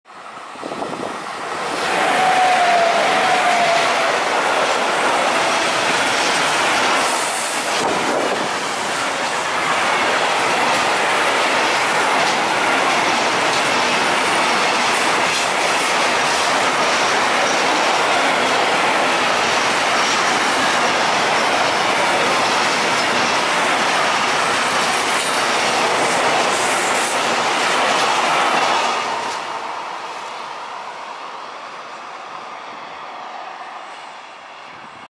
PRIMO SERVIZIO MERCI PER LA NUOVA EUROSPRINTER EF64F4-114 DI AKIEM!
Primo servizio merci per la nuova Eurosprinter ES64F4-114 di Akiem in livrea argento e grigio scuro con dettagli rossi, traino di un treno merci TEC in coppia con un altra Eurosprinter la ES64F4-087